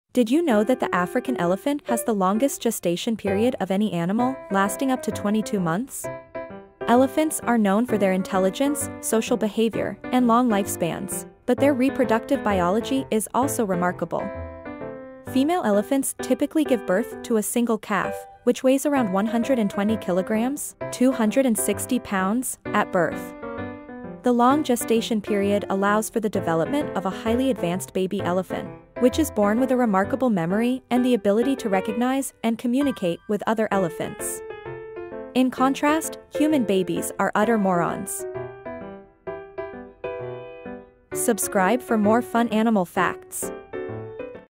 Elephants are Pregnant for HOW Long?? - Animal Facts
Did you know that the African elephant has the longest gestation period of any animal, lasting up to 22 months? Elephants are known for their intelligence, social behavior, and long lifespans, but their reproductive biology is also remarkable. Female elephants typically give birth to a single calf, which weighs around 120 kilograms (260 pounds) at birth. The long gestation period allows for the development of a highly advanced baby elephant, which is born with a remarkable memory and the ability to recognize and communicate with other elephants. In contrast, human babies are utter morons.